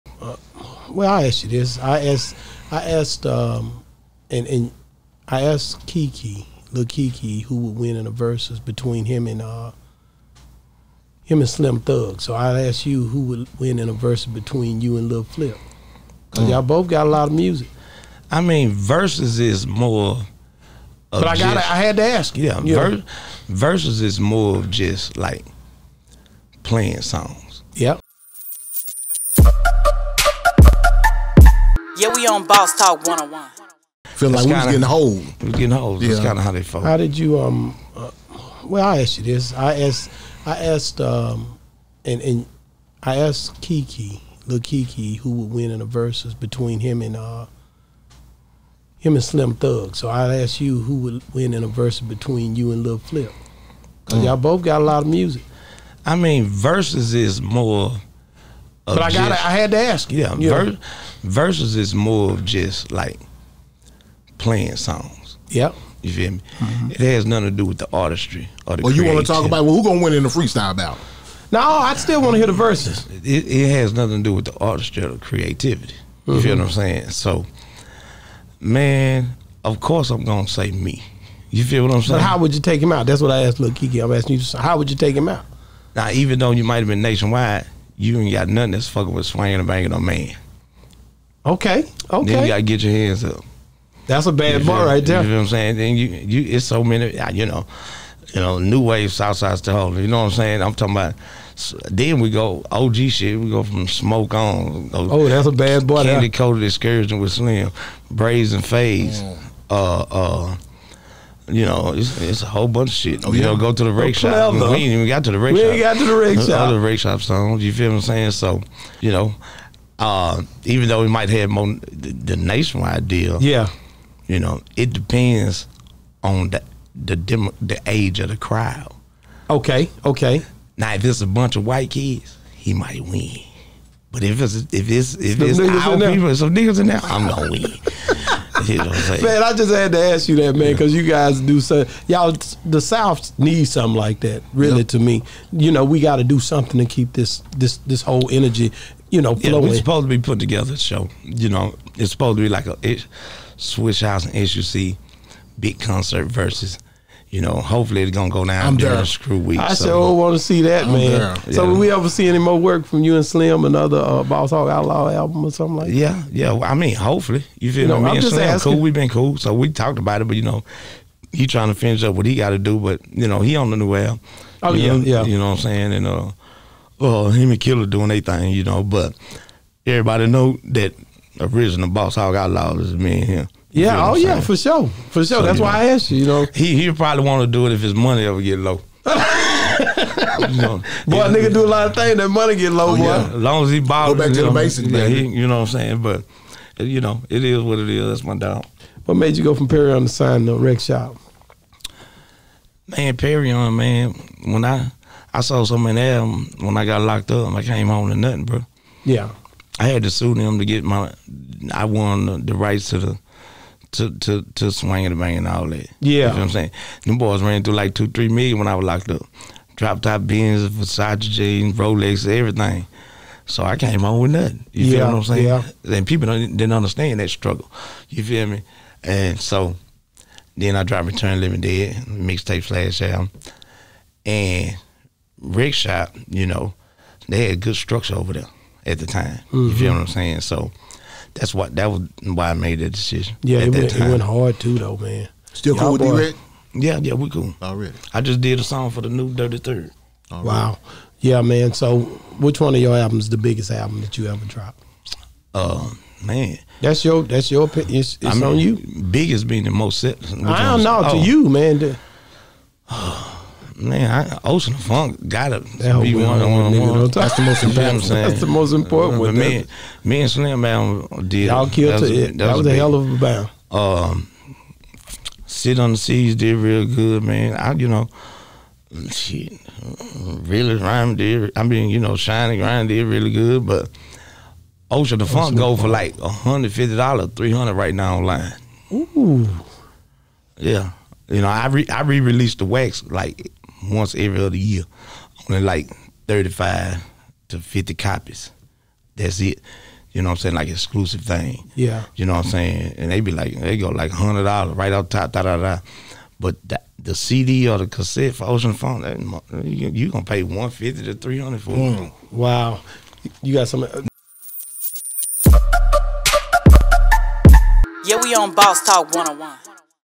ESG on Verzuz with Flip Who Would Win? Will there ever be another BOSS HOGG OUTLAWZ Project (Part 5)
E.S.G on a Verzuz between him and Lil Flip who would win?

Will there ever be another Boss Hogg Outlawz?